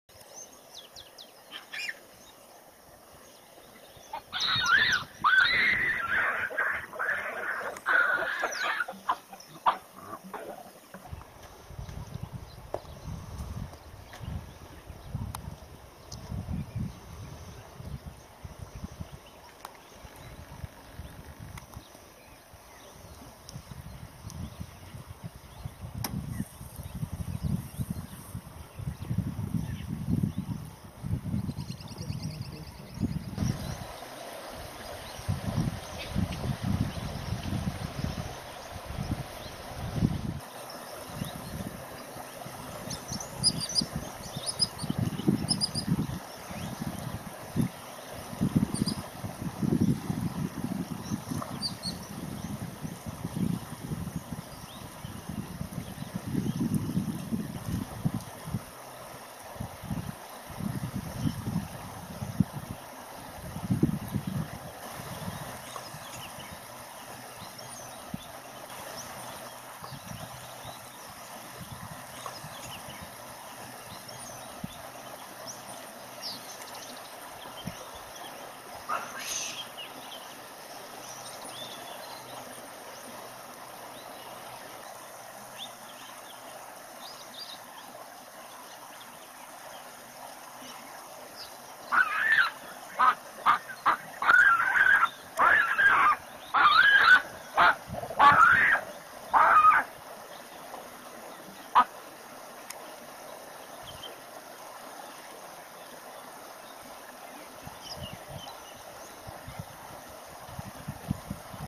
Baboons by the river
Normally seen in the open savanah, up trees, and various other places, but they do like to beside rivers.